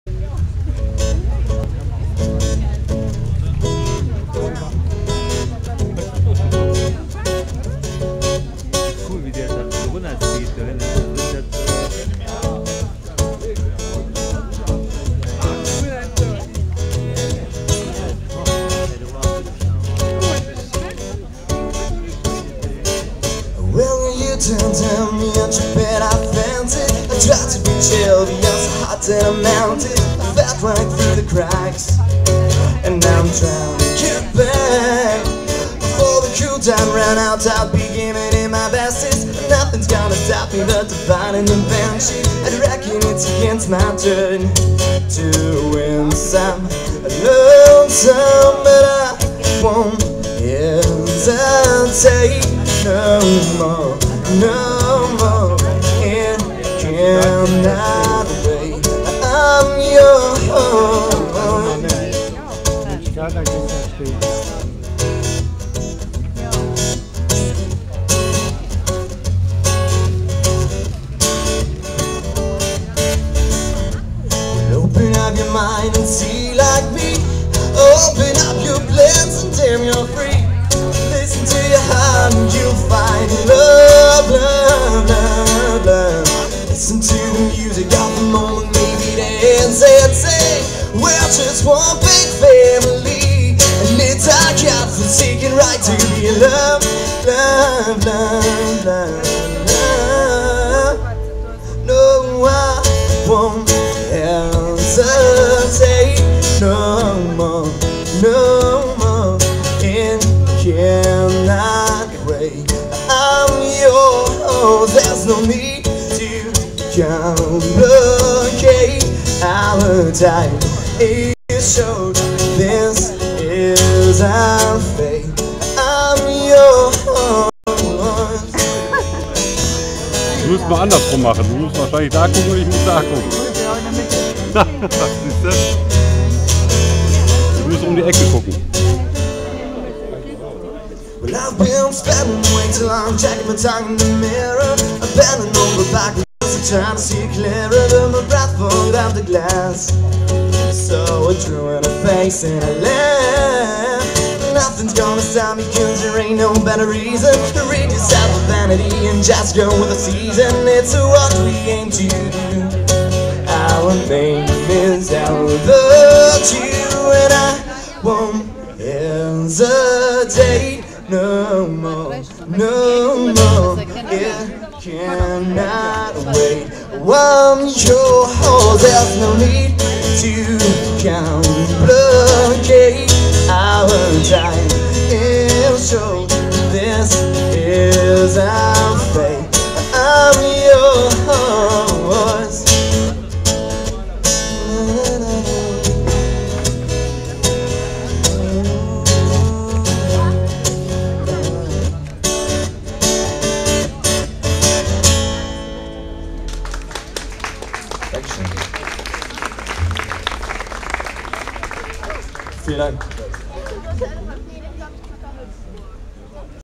XXL 3D Strassenmalerei in Sankt Wendel mit FreddArt Streetpainting
XXL 3D Strassenmalerei Projekt von Marion Ruthardt in Zusammenarbeit mit FreddArt Streetpainting, Gregor Wosik und Wilfried Berg. Tatkräftig unterstützt von den Helfern István Bába und Barbara Radler. Im Auftrag von der Kreisstadt Sankt Wendel.